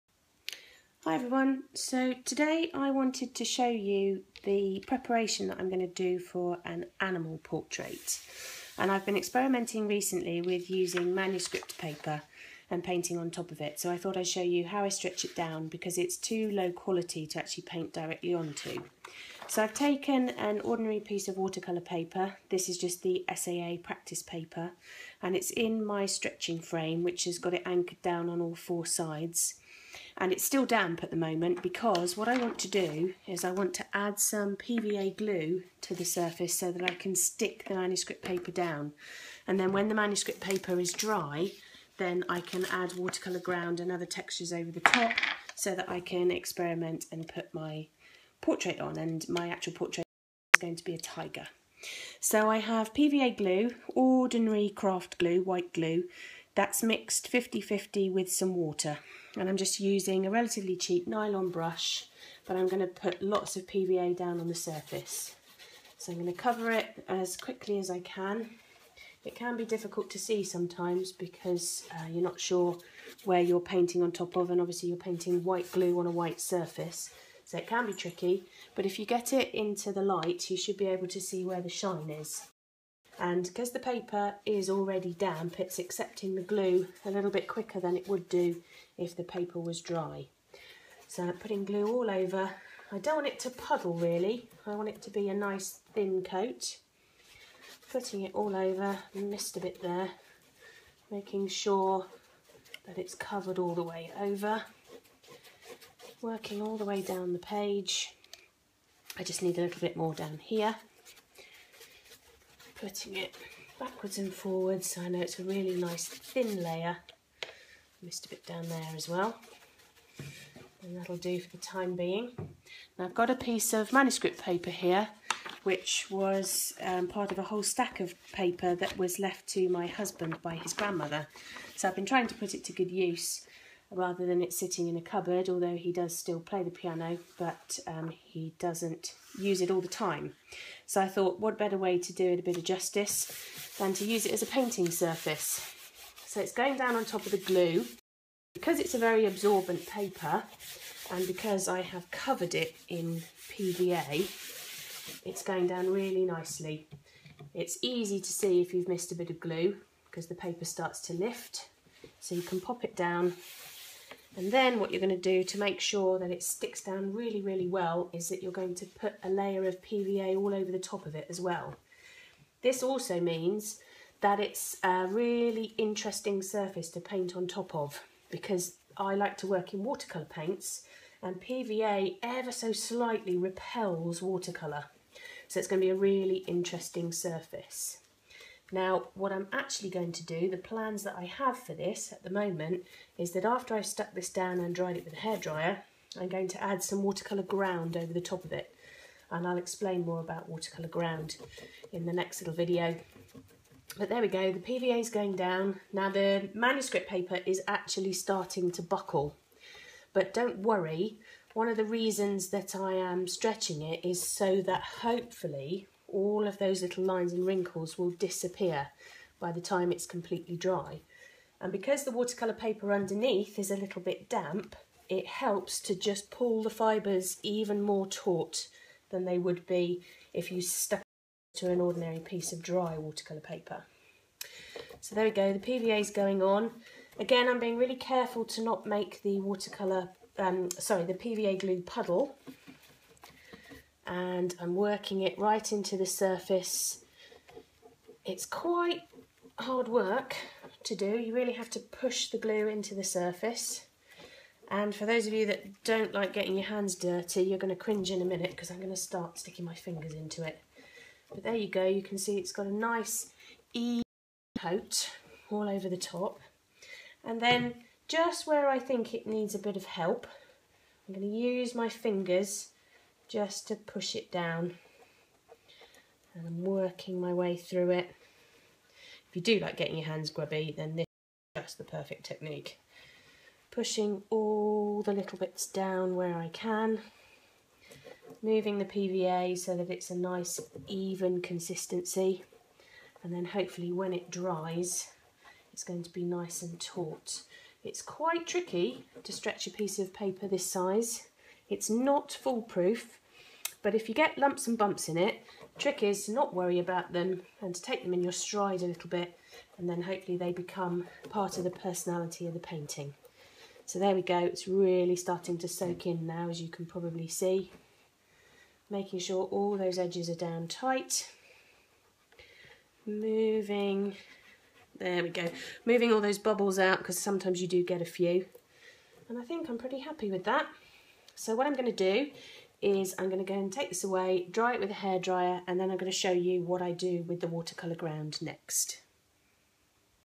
Stretching Manuscript Paper
Preparing my surface for a demonstration by showing how I stretch manuscript paper on to watercolour paper.